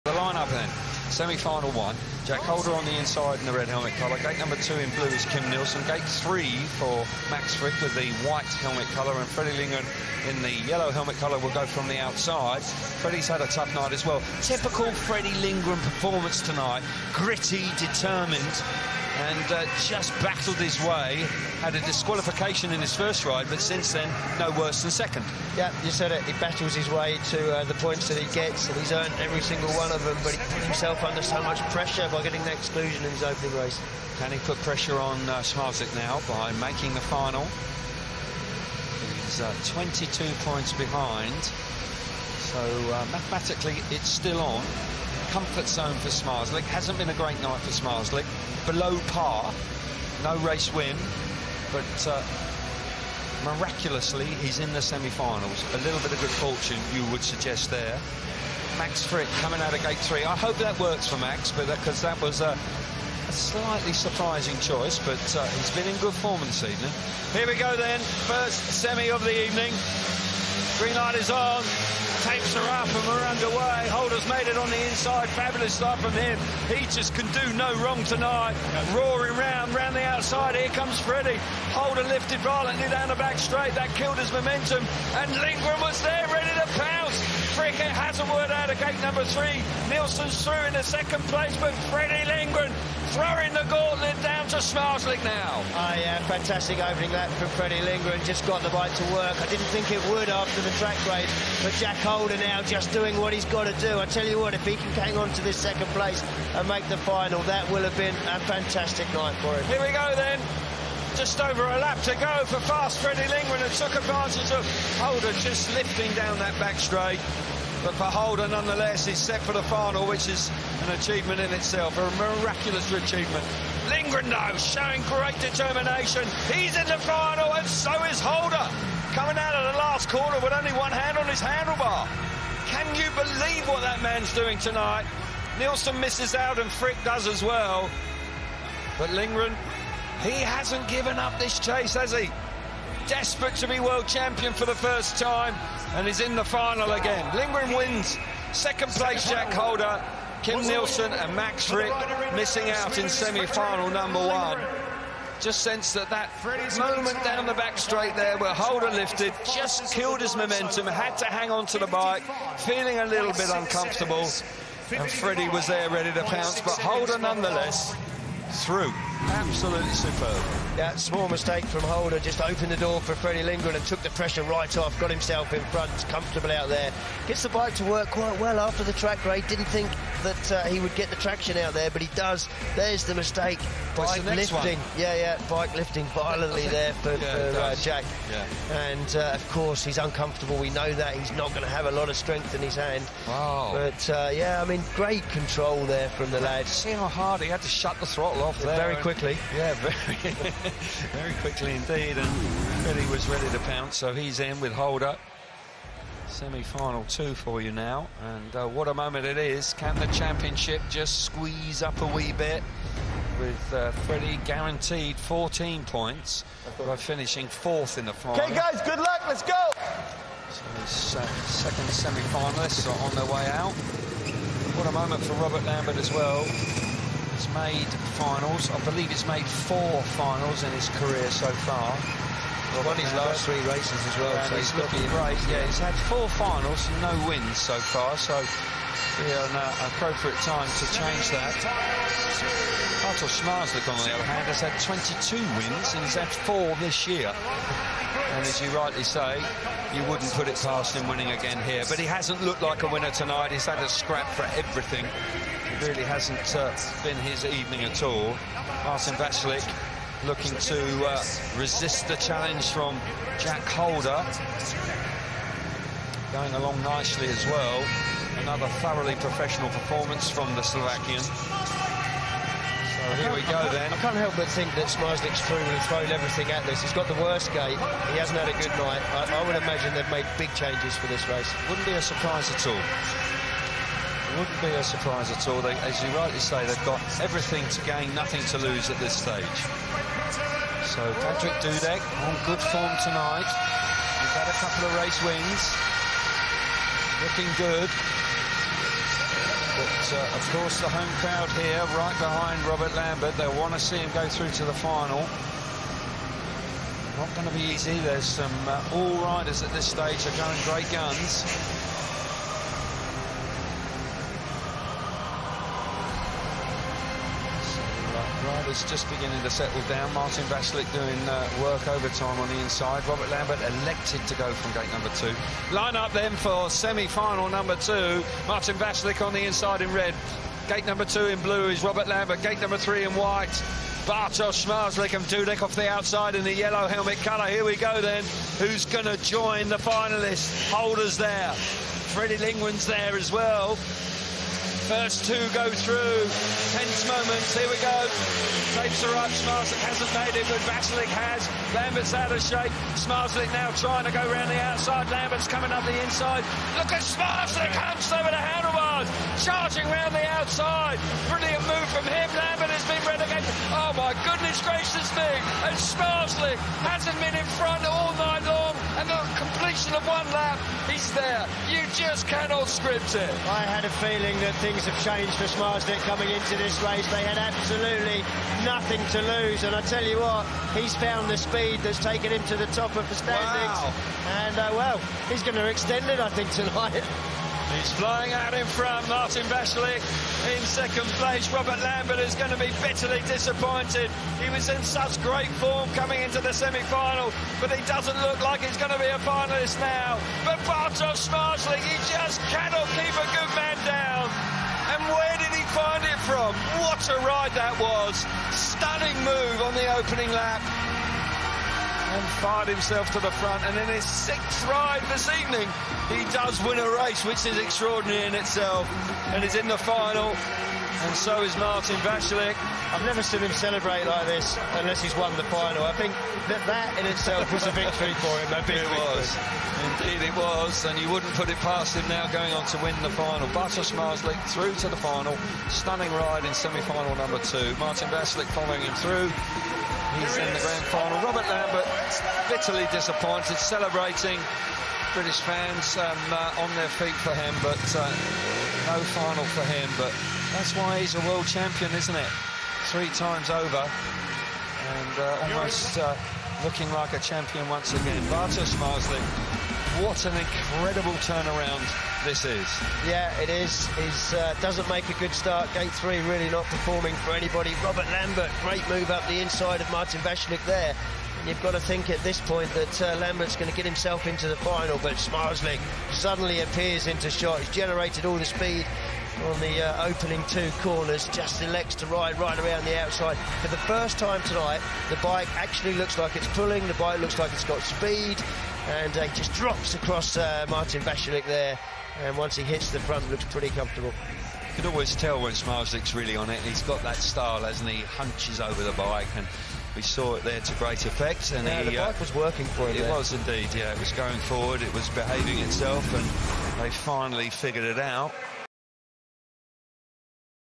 Ride or Die time! 🔥 Both Semi-Finals BritishSGP | FIM Speedway Grand Prix
2 epic battles for those 4 places in the final! ⚔️

Watch both Semi-Finals from the BritishSGP again now! 💥